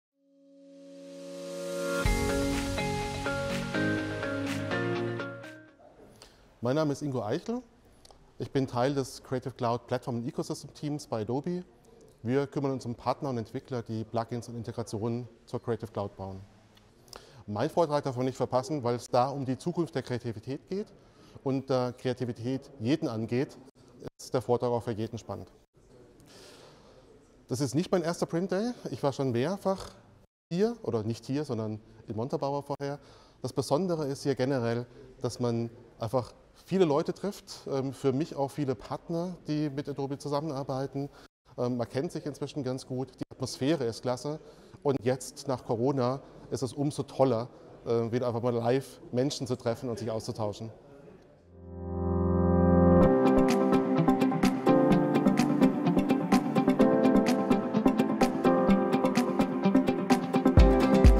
10. priint:day | Interview mit Ingo Eichel von Adobe
Ingo Eichel vom Creative Cloud Plattform Ecosystems Team hat in seiner Präsentation über Kreativität und die damit einhergehenden Arbeitsweisen bei Adobe gesprochen. In dem kurzen Interview erklärt er, warum man seinen Vortrag nicht verpassen sollte und er alle zwei Jahre beim priint:day dabei ist.